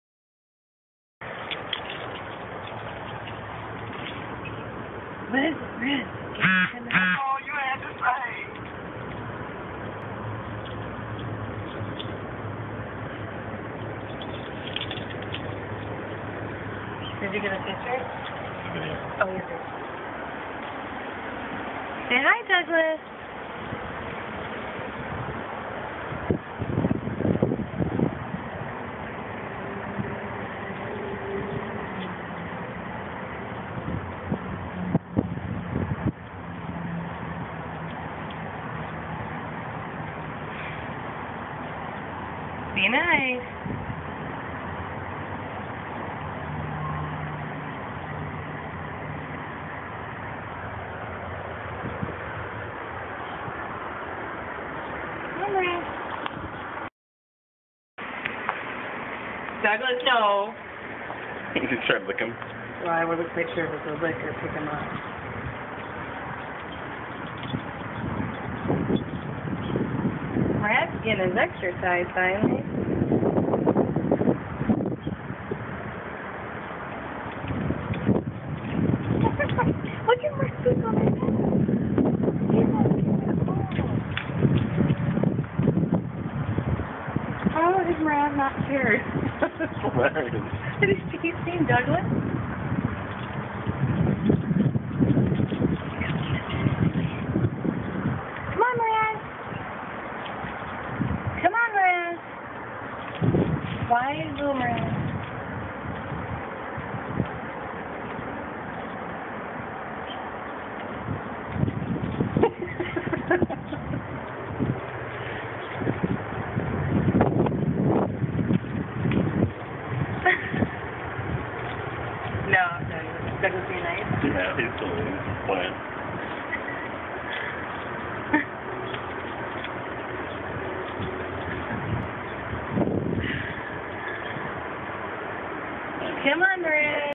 Douglas & Mraz
Douglas is a dog. Mraz is a turtle.